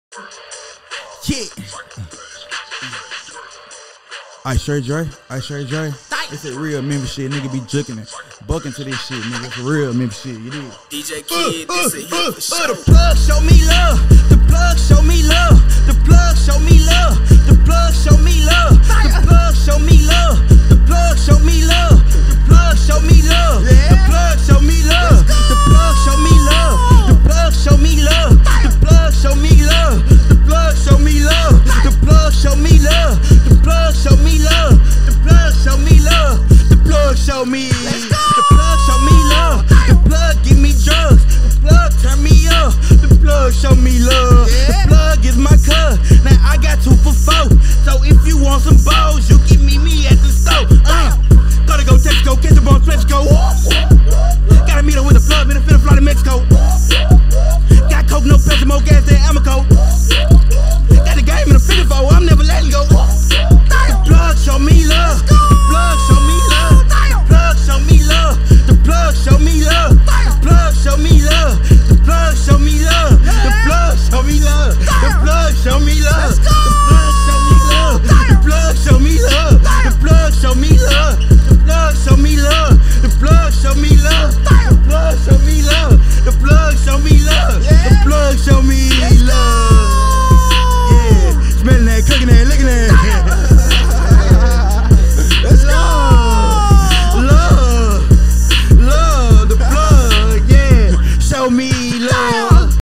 East Memphis Rapper IceTrayDre Stops by Drops Hot Freestyle on Famous Animal Tv
East Memphis Rapper IceTrayDre Stops by Drops Hot Freestyle on FamousAnimalTv